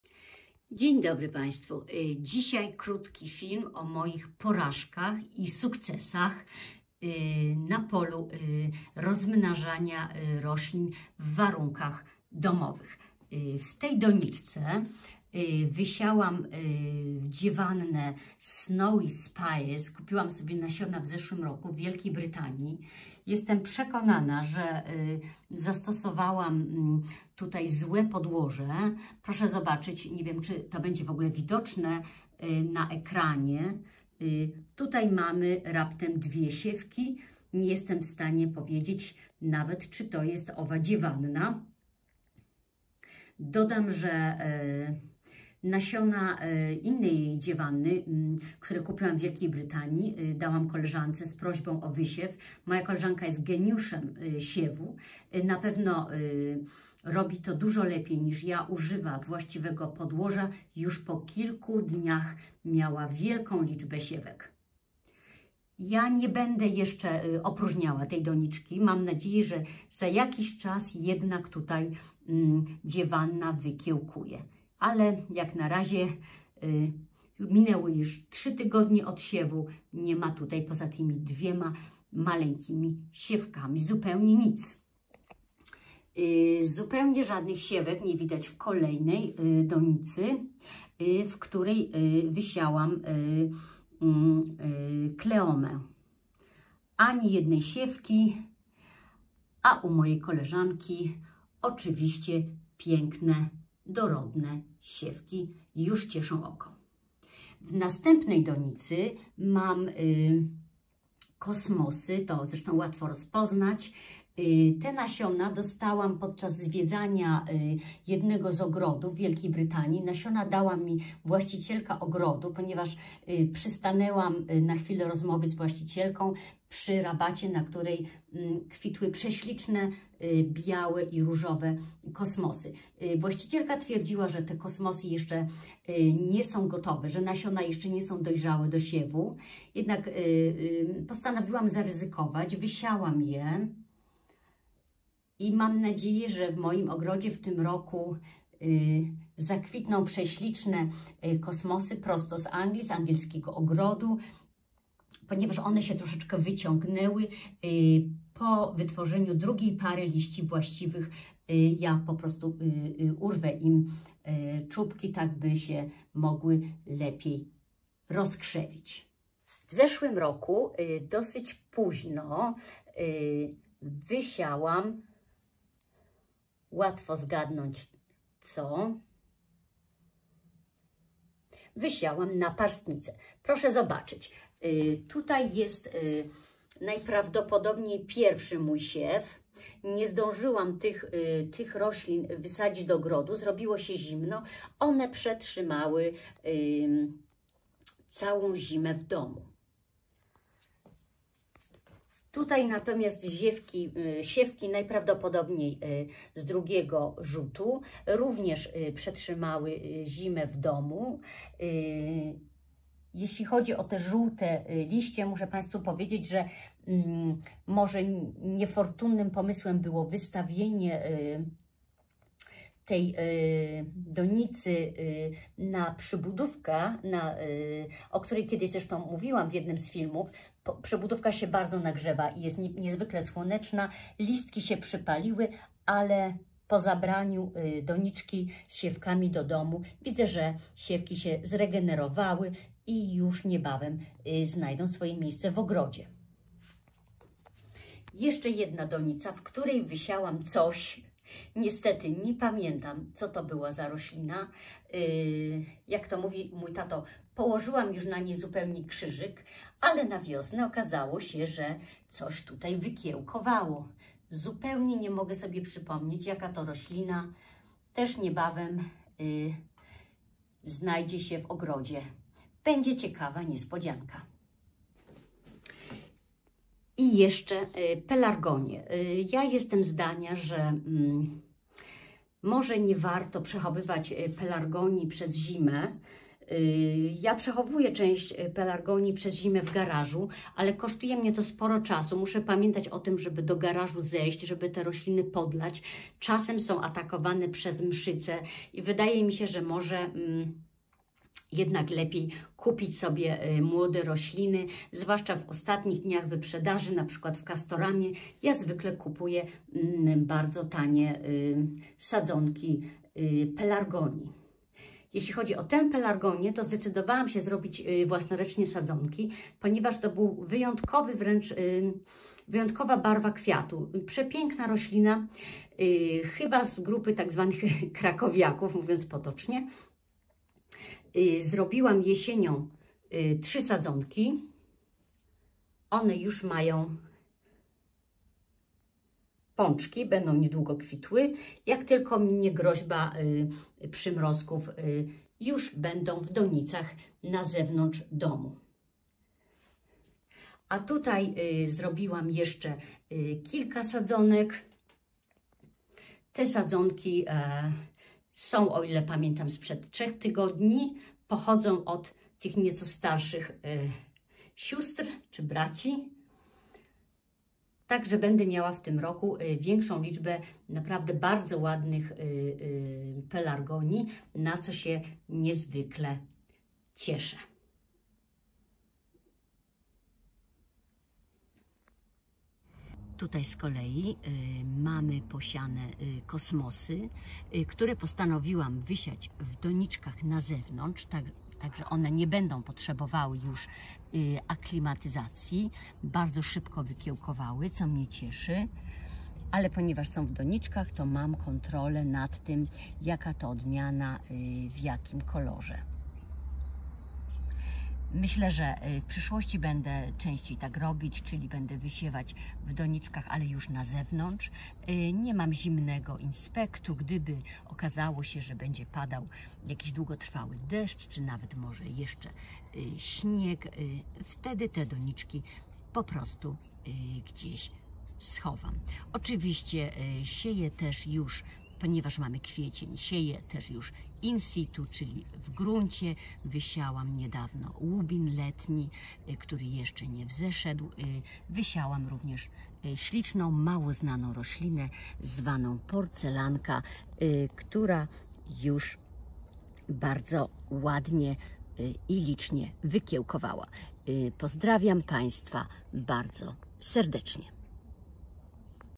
PORAŻKI i SUKCESY w rozmnażaniu roślin
Opowiadam o moich ostatnich sukcesach i porażkach w rozmnażaniu roślin przez siew (generatywnym) oraz w robieniu sadzonek pędowych (sposobie wegetatywnym).
Ostatnie minuty filmu nakręciłam za zewnątrz, ze względu na sąsiadów mówię nieco ciszej.
My channel in English @gardensenglishanddance